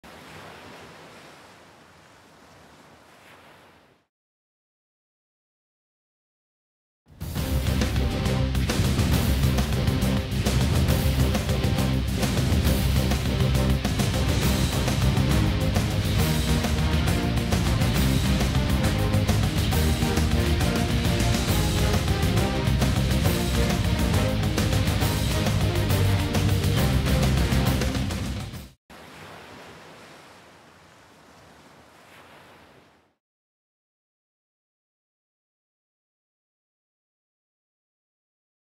Best Waves: Bonga Perkins 2010
Bonga Perkins's Best Wave from the He'e Nalu TNTV Sapinus Pro, presented by Air Tahiti Nui